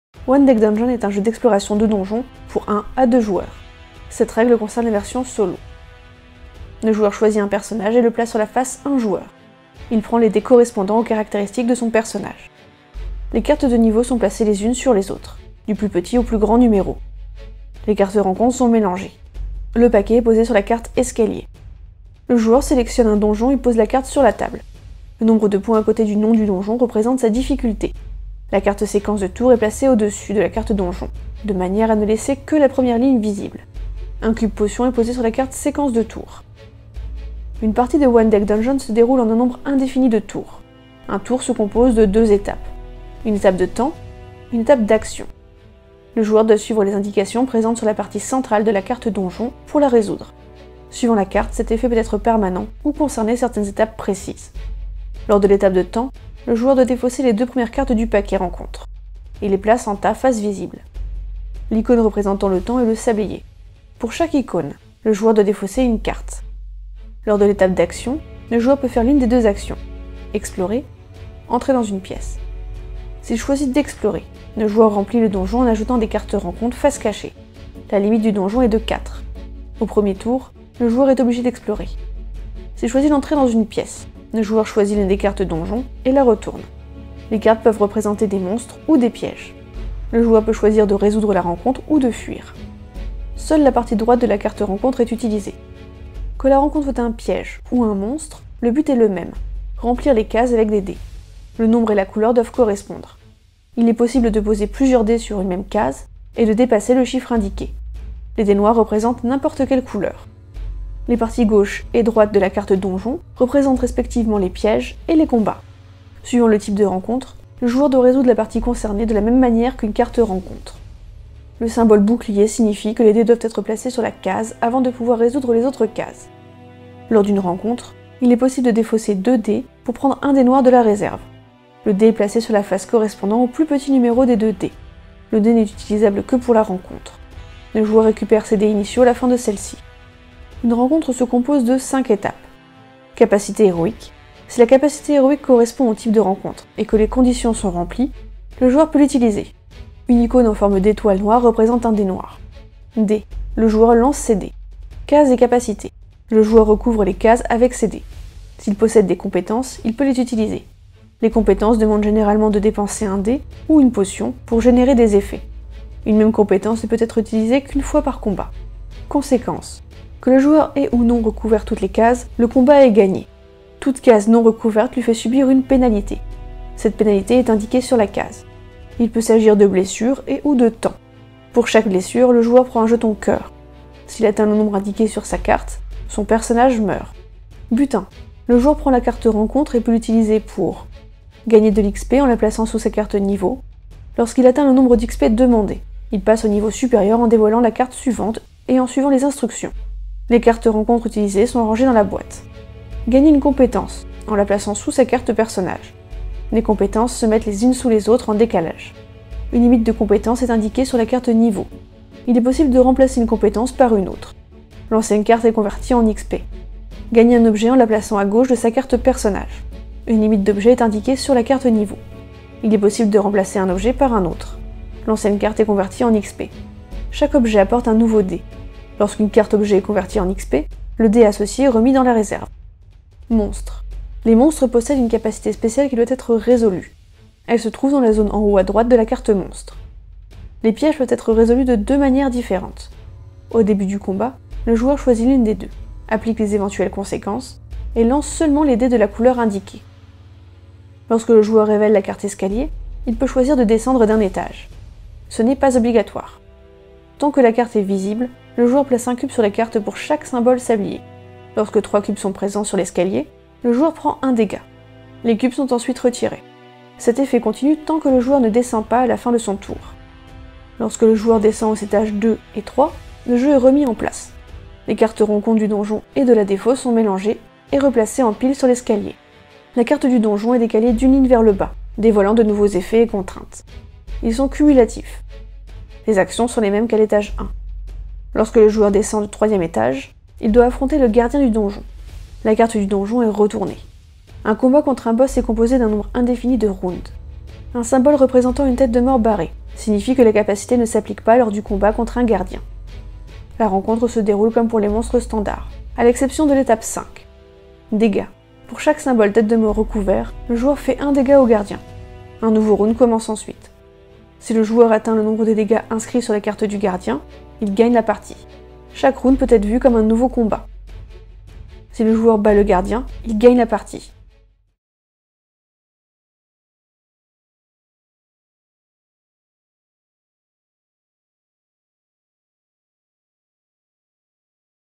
One Deck Dungeon - Règle Express
One Deck Dungeon est un jeu d’exploration de donjon et de dés pour 1 à 2 joueurs.

Attention, quelques erreurs de règles :
1:45 : il est possible de poser plusieurs dés sur les grandes cases. Les petites ne doivent en contenir qu'un.
3:24 : les compétences Potion sont placées sous la carte niveau et ne comptent pas dans la limite. S'il s'agit de la première compétence Potion portant ce nom, le joueur gagne un cube potion.

Les icones ont été faites par Smashicons (sablier), Bogdan Rosu (refresh), Smashicons (porte, personnage), srip (boussole), Freepik (armes), et téléchargées